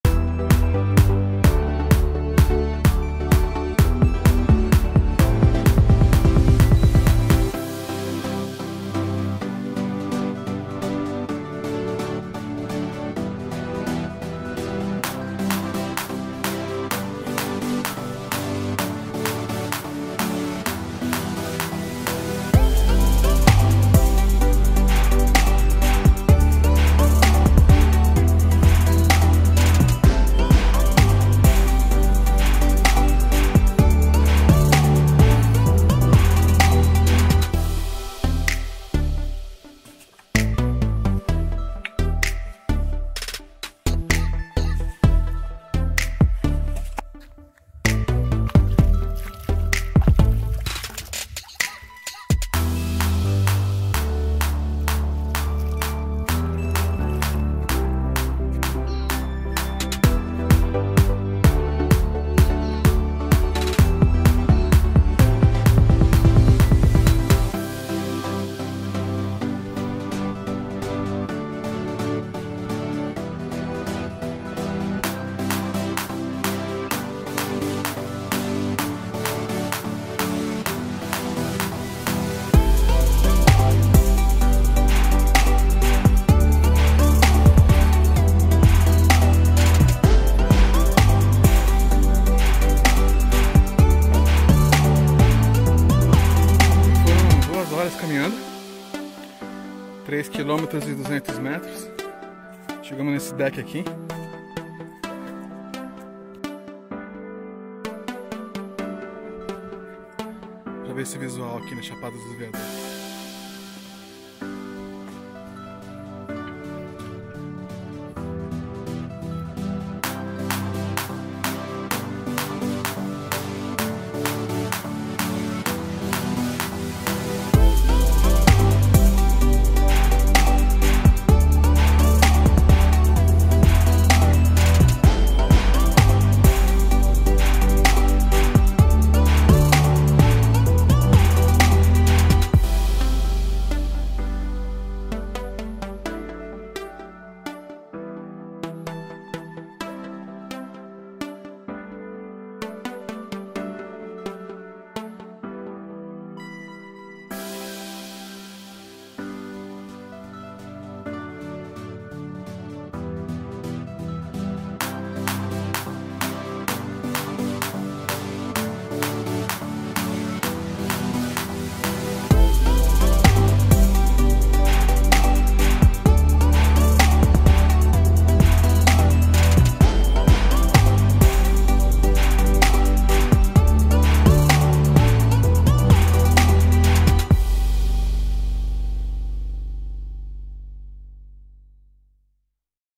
Janela do abismo - Chapada dos Veadeiros Goiás
Um pouco de como é a Trilha até a Janela do Abismo na Chapada dos Veadeiros em São Jorge/Goiás

Trilha total de 7km, levamos 5 horas para ir e voltar contando com a longa parada na cachoeira na volta para relaxar.

Trilha com trechos de moderado acesso.

Mas valeu cada passo